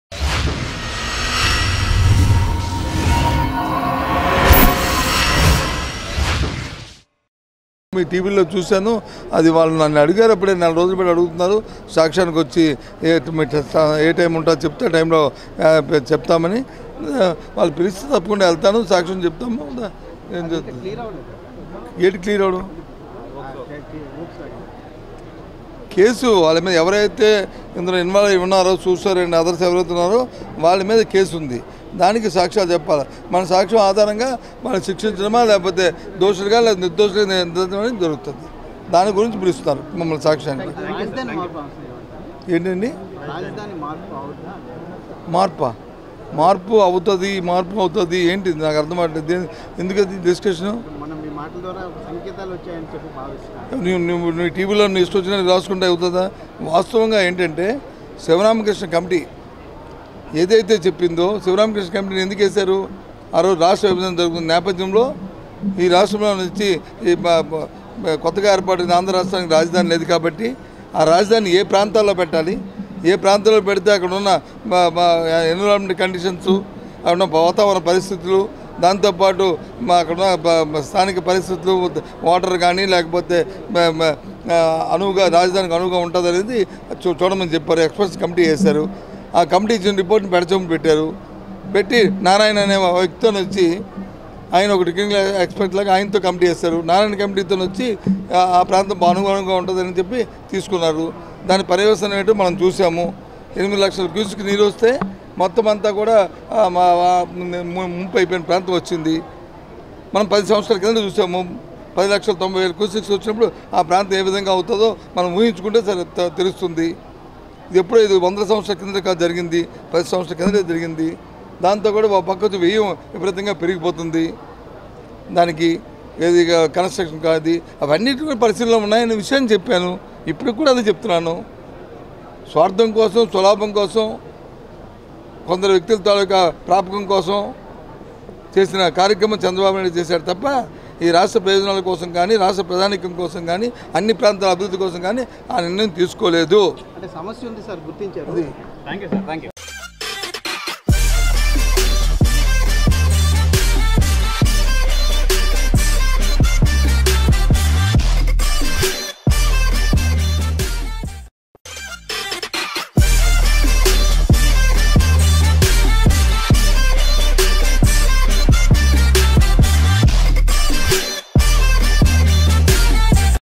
Minister Botsa Satyanarayana Revels Shocking About AP Capital City || AP CM YS Jagan || Sunray Media
Minister Botsa Satyanarayana Revels Shocking About AP New Capital City || AP CM YS Jagan || Sunray Media